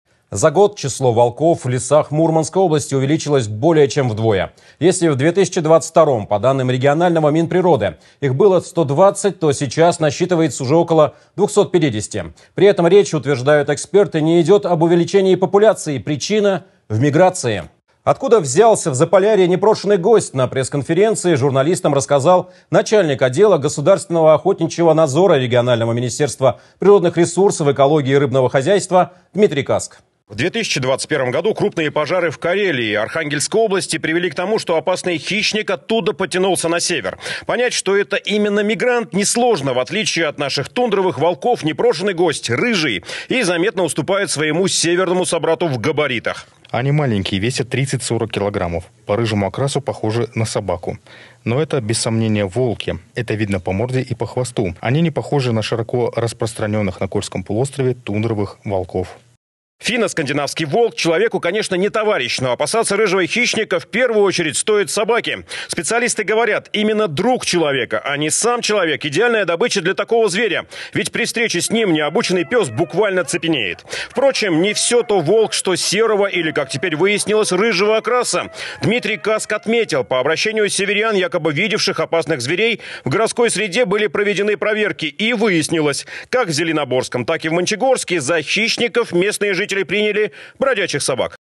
Число волков в лесах Мурманской области увеличилось более чем вдвое
За год число волков в лесах Мурманской области увеличилось более чем вдвое. Причина – в миграции. Подробнее на пресс-конференции журналистам рассказал начальник отдела государственного охотничьего надзора регионального министерства природных ресурсов, экологии и рыбного хозяйства Дмитрий Каск.